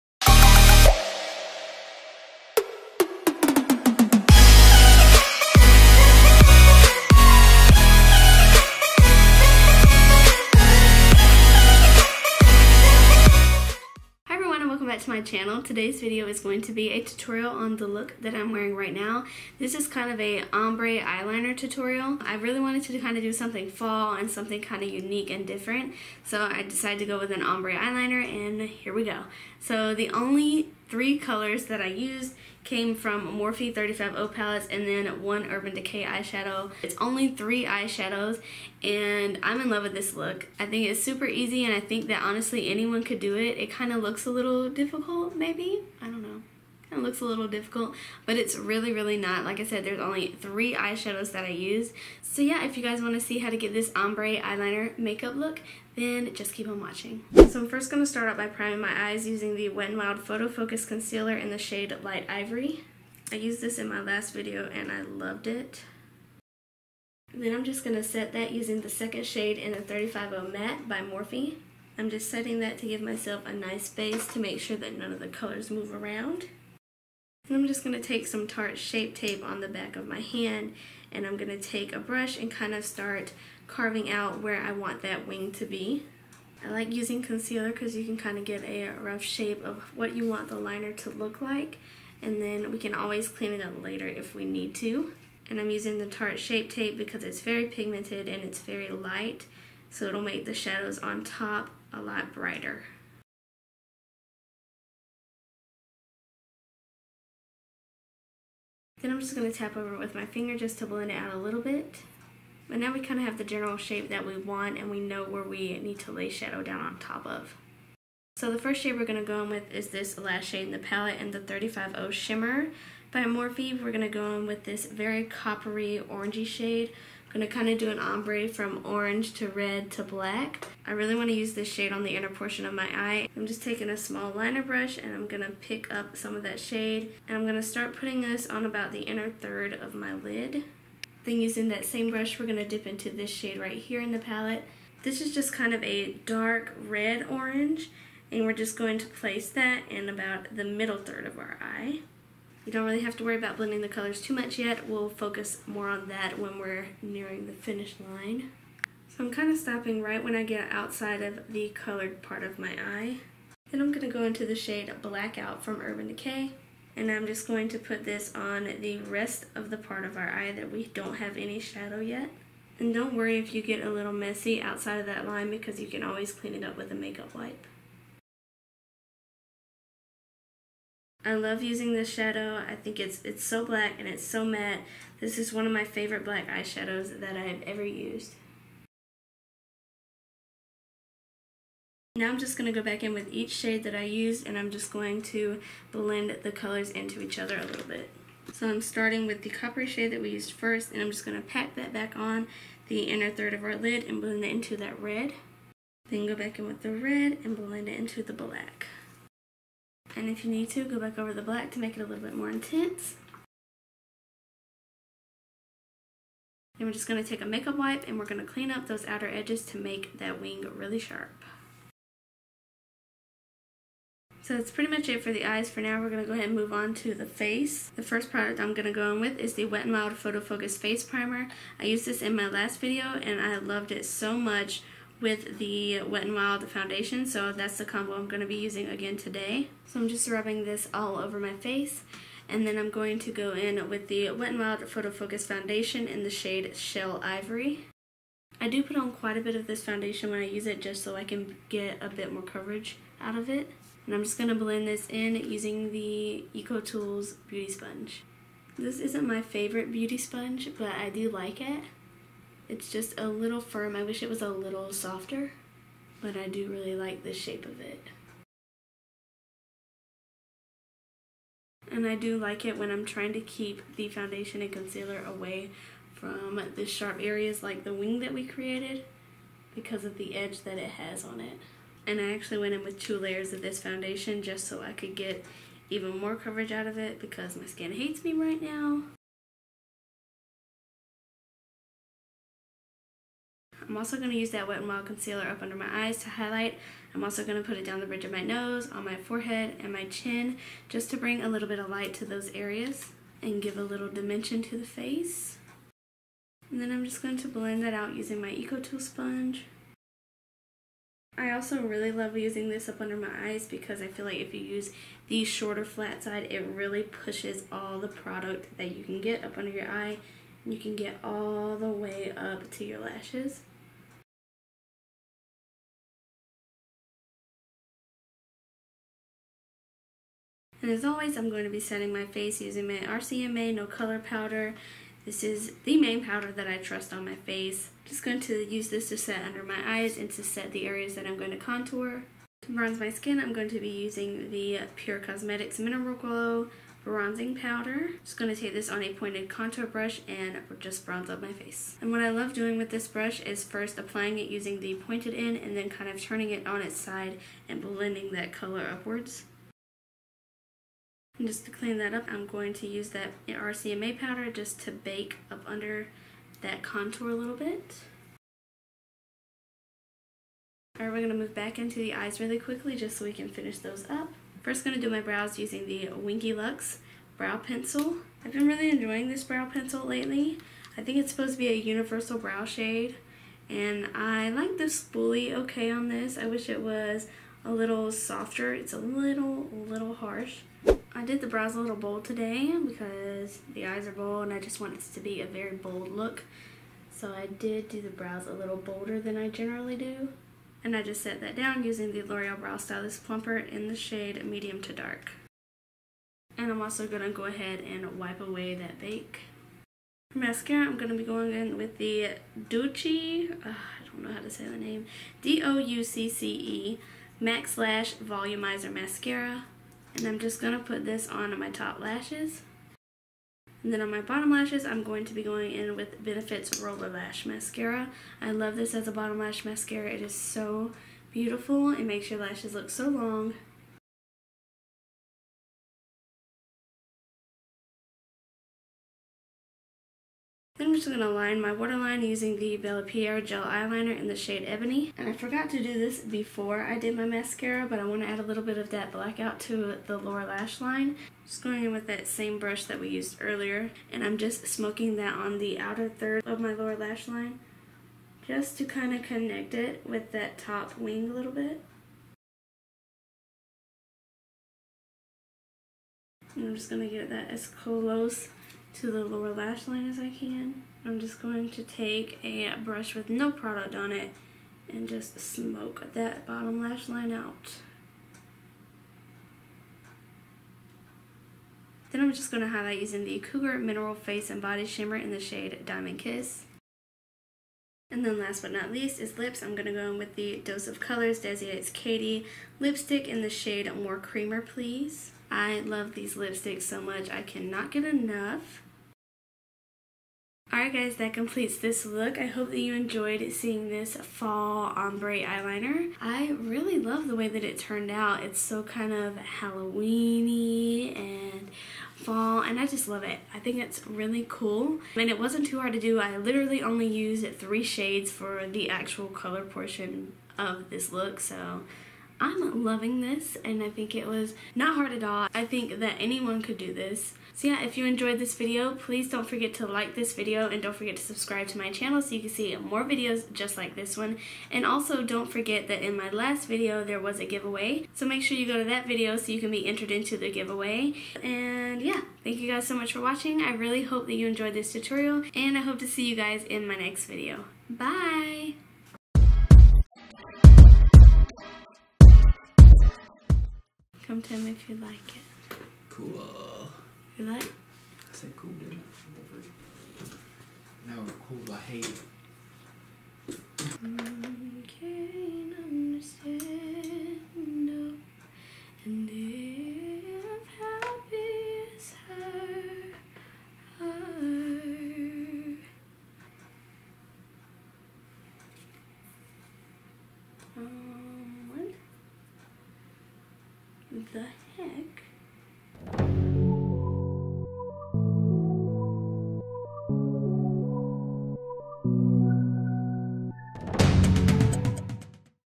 OMBRÉ EYELINER TUTORIAL | Fall Makeup Look!
Hi everyone! Today's video is a tutorial on this ombre eyeliner look! (Check out my IG and Twitter linked below to see more close ups!) This look is perfect for fall and I am absolutely in love with it! Annnnnnnnnd, I only had to use three eyeshadows! I know it might look a little difficult, but it's really very easy. Also, don't forget to check out my last video and enter into my Wet N Wild favorites giveaway! XOXO

Products used:
 Morphe 35O Matte
 Morphe 35O Shimmer
 Urban Decay Blackout Eyeshadow
 Wet N Wild Face Primer
 Wet N Wild Photofocus Foundation in Shell Ivory
 Wet N Wild Photofocus Concealer in Light Ivory
 Pur Cosmetics Mineral Glow Bronzing Powder
 RCMA No Color Powder
 Winky Lux Universal Brow Pencil
 Loreal Brow Stylist Plumper Shade Medium to Dark
 Cougar Cosmetics Diamond Kiss Highlighter
 DESIxKATY Dose of Colors Lipstick in Shade More Creamer
Please
 Doucce Maxlash Volumizer Mascara
 Bella Pierre Gel Liner Shade Ebony